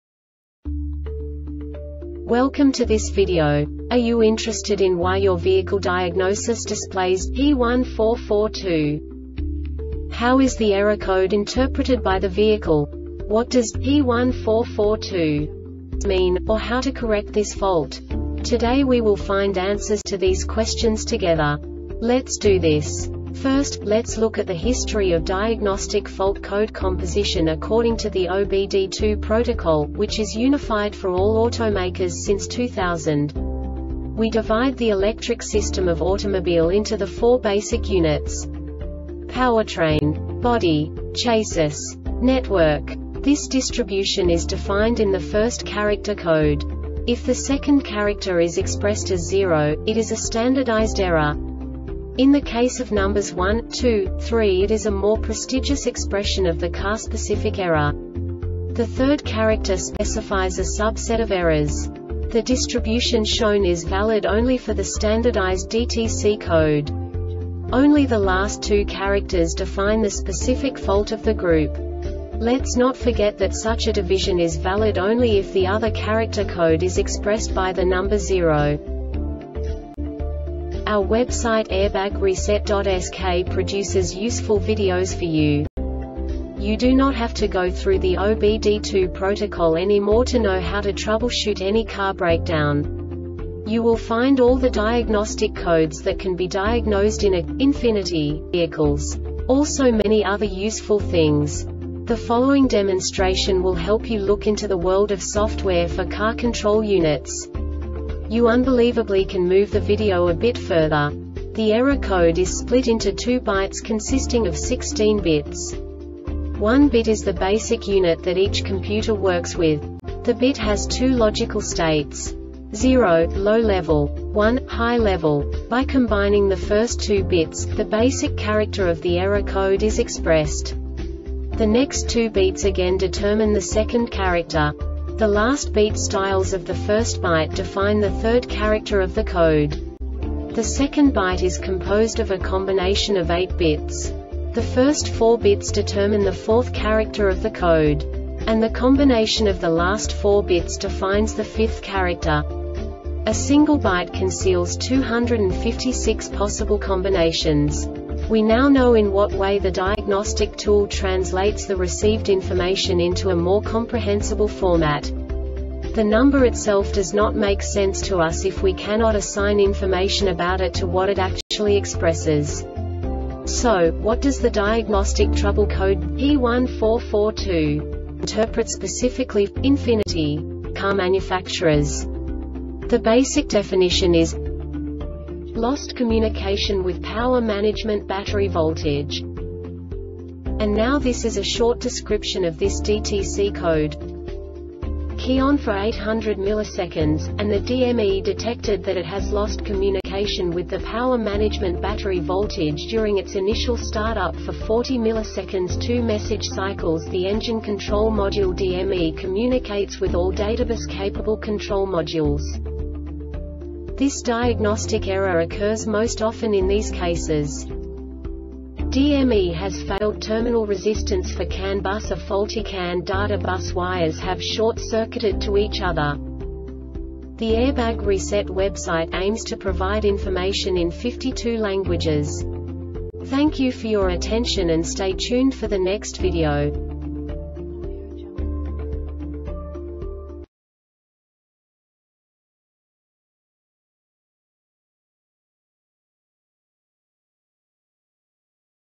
DTC Ford P1442 Short Explanation
Contents:
0:21 Basic DTC analysis according to OBD2 protocol standard.
1:48 Insight into programming
2:58 A diagnostically understandable description of the Diagnostic Trouble Code
3:05 Basic error definition

Make:
Ford
Code:
P1442
Definition:
Lost Communication With Power Management Battery Voltage
Description: